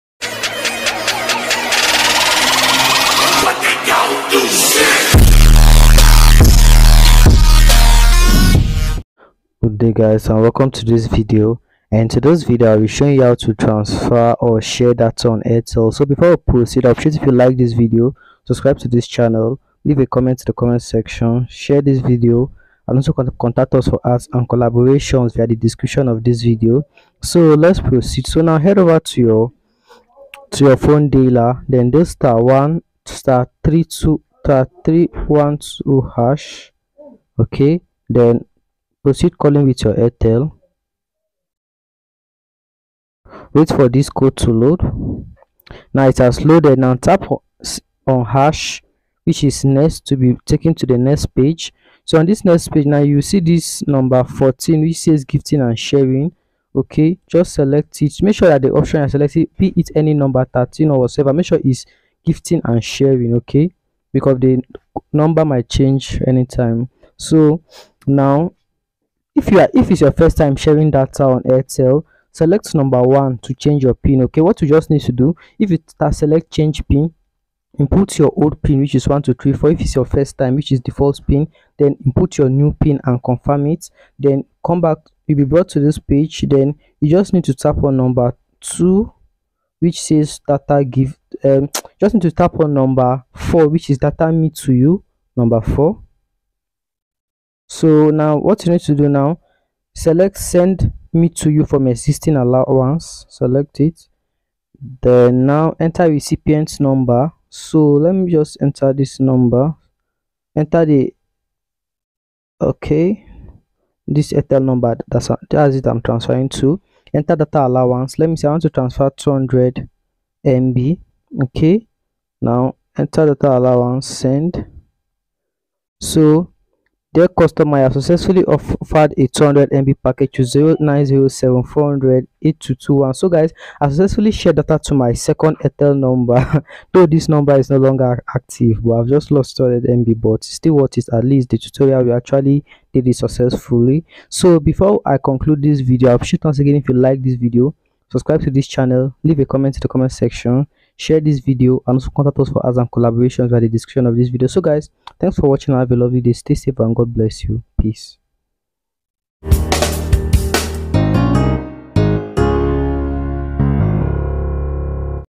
How To Share Data On Airtel | Transfer Data On Airtel
Are you looking for a hassle-free way to share data on Airtel? Look no further! In this comprehensive tutorial, we will walk you through the easy steps to successfully share your data with friends and family on the Airtel network. Sharing data on Airtel has never been easier. With our step-by-step guide, you'll become an expert in no time. Whether you want to share your data with a loved one or simply transfer data from one device to another, we have got you covered.

🎯 By the end of this tutorial, you will have a thorough understanding of how to share data on Airtel seamlessly, bringing you closer to your loved ones and enabling uninterrupted internet access for all. Don't miss out on the opportunity to unlock the full potential of data sharing on Airtel.

?sub_confirmation=1

Twitter Handle

🔑 Related Keywords 🔑